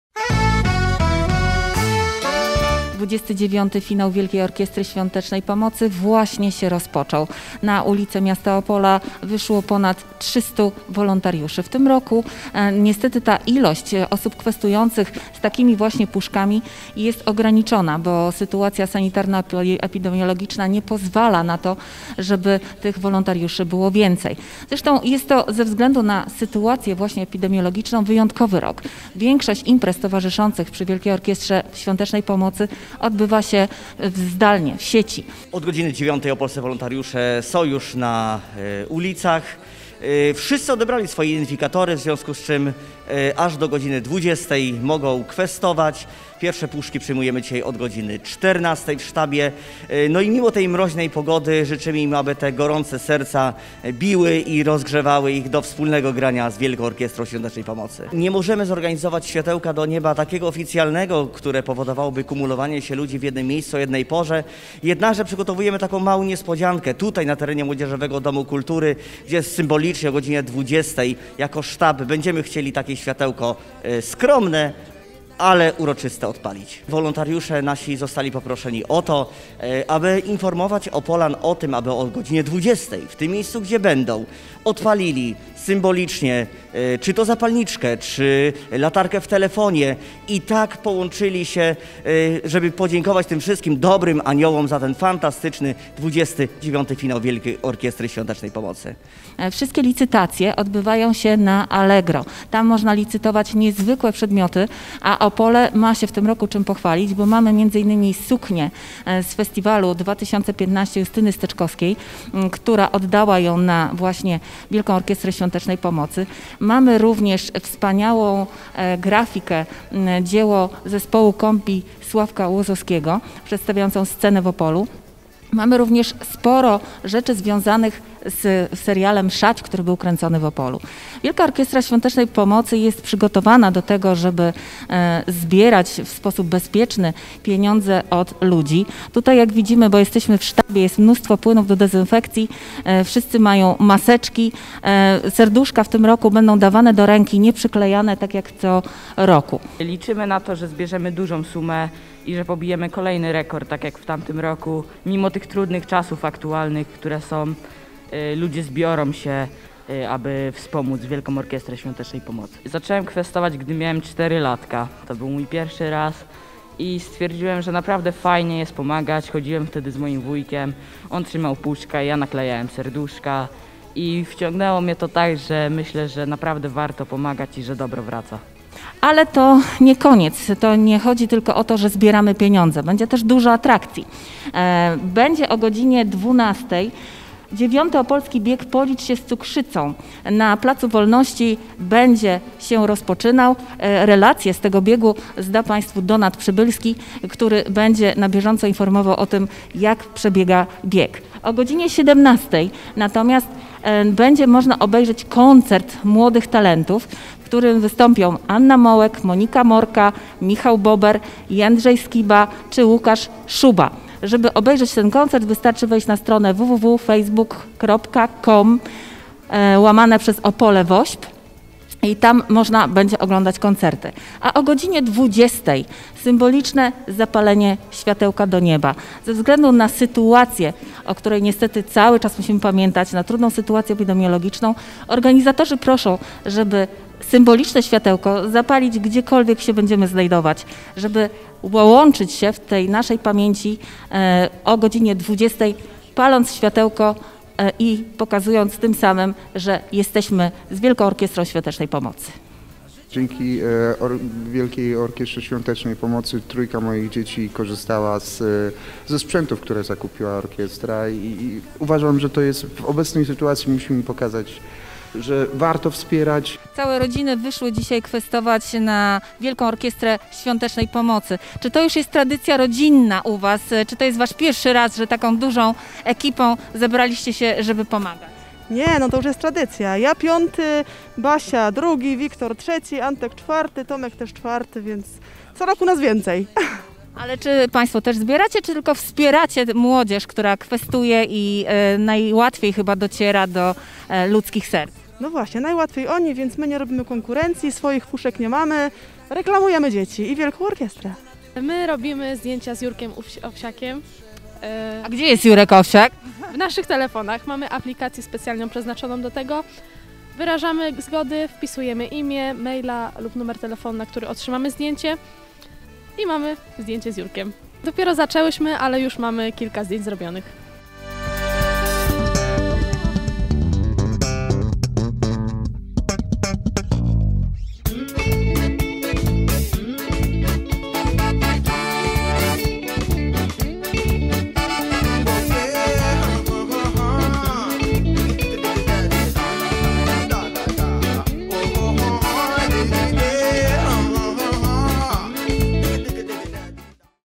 Ruszył 29 finał WOŚP w Opolu.
Od rana wolontariusze WOŚP kwestują na ulicach. Pomimo mrozu całe rodziny zbierają datki a darczyńców nie brakuje. Nowością tegorocznej orkiestry są między innymi zdjęcia z wirtualnym Jurkiem Owsiakiem robione przez wolontariuszy.